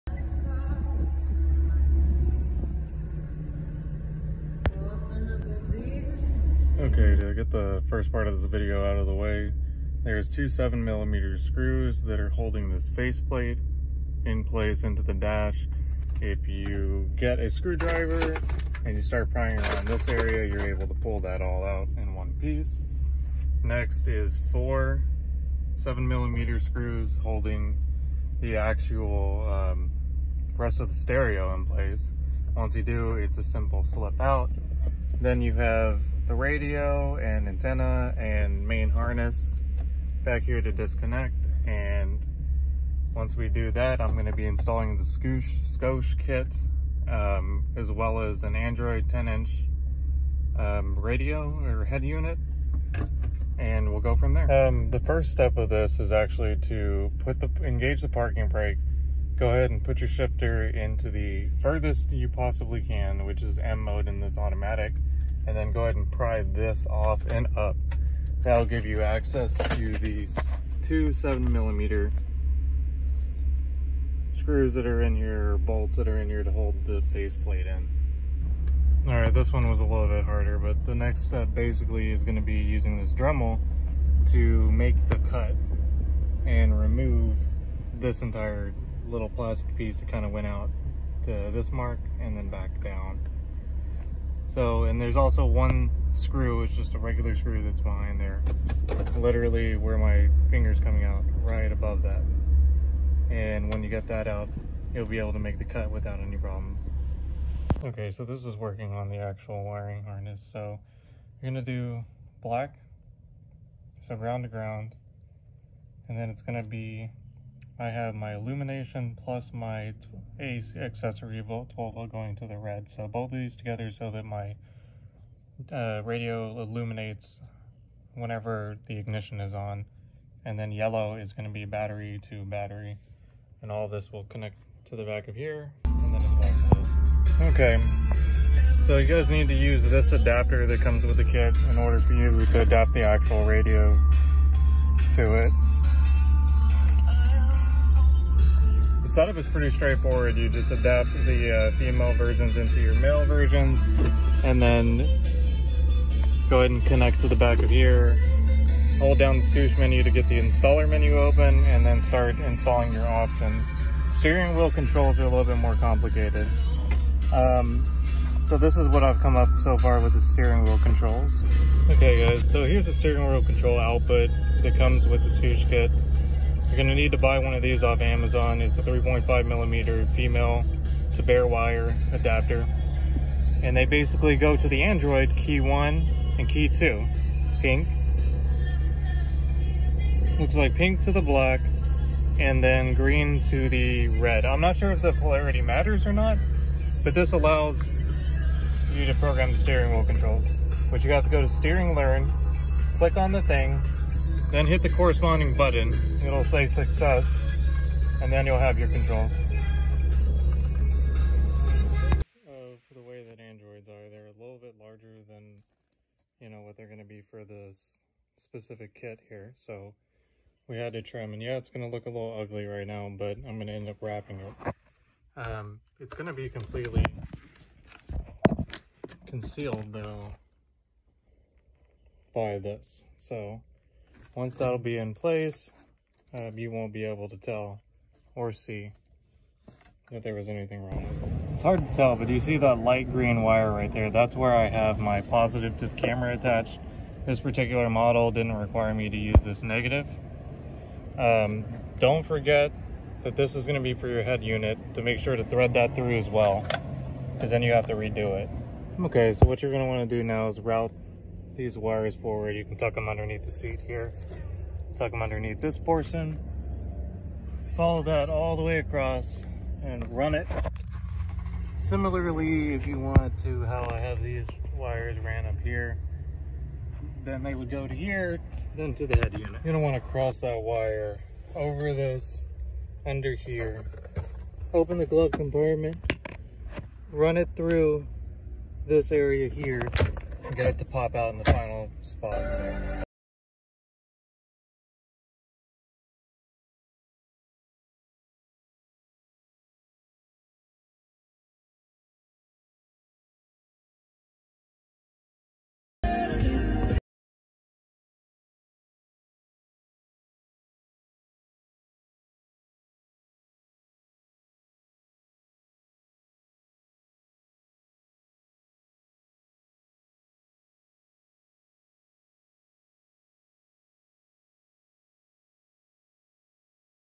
2010-15 camaro Radio upgrade Android 10.1 inch with YouTube and scosche camaro 2.0 dash kit install
This is a overview of my DIY install of the scosche camaro dash kit 2.0 and 10.1 inch android head unit with an aftermarket back up camera